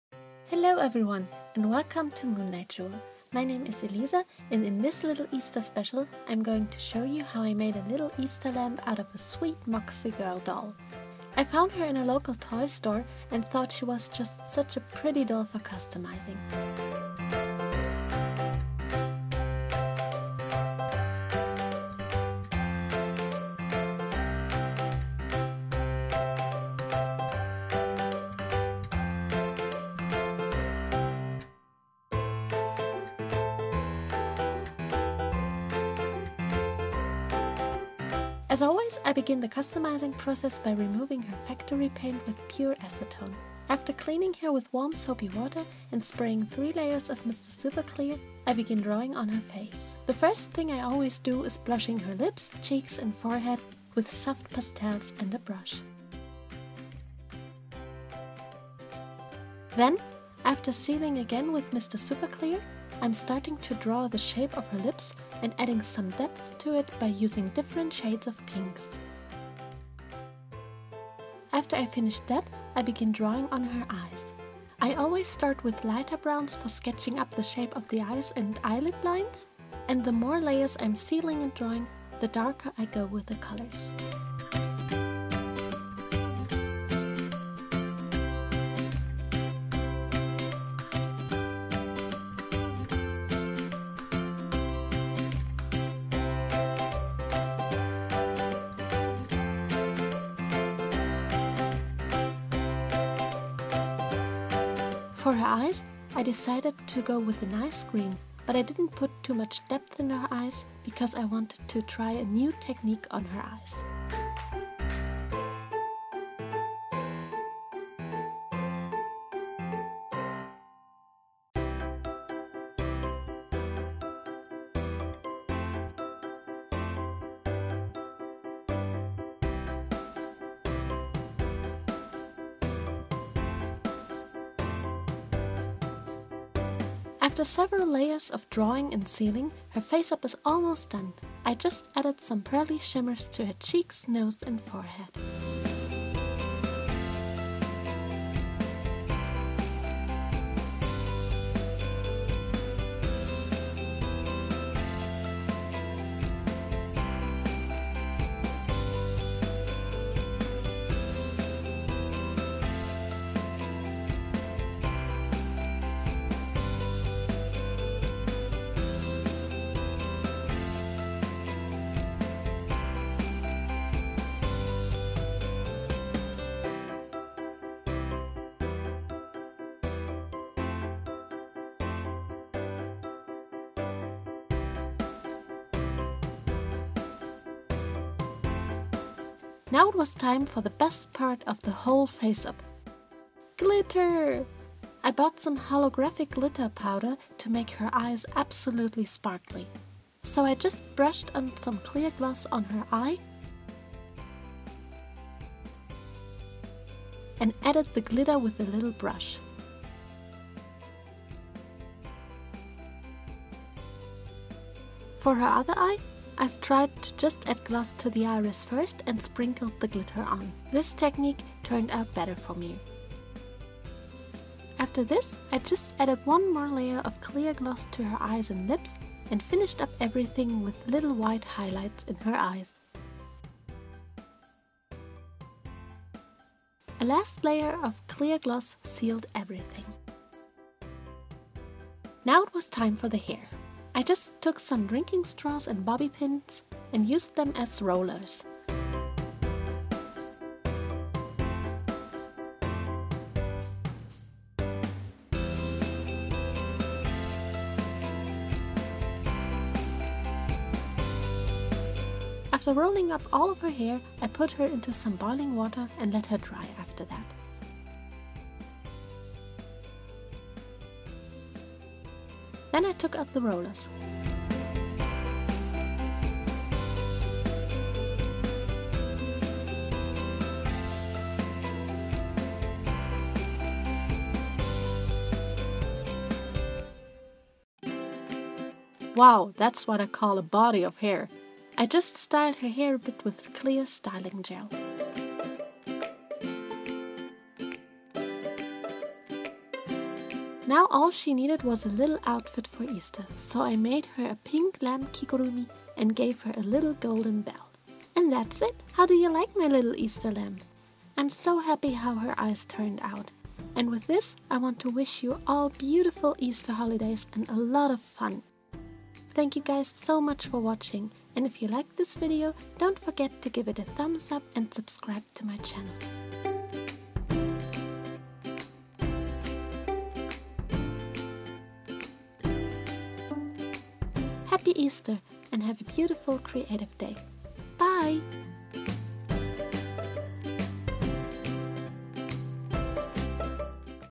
☽ Moonlight Jewel ☾ Easter Special Repaint Moxie Girlz
This is my little Easter Special. I had some custom fun with my Moxie Girl doll and transformed her into a sweet little Easter lamb with sparkling eyes!

Enjoy watching and stay creative! ❤

☽ LINKS ☾

☽ MATERIALS ☾

Polychromos Pencils - Faber Castell
Soft Pastels - Toison D'or
Mr. Super Clear UV Flat
Acrylic paint
Pearlescent powders
Kiwi Hair
Polymer Clay (FIMO)
Glitter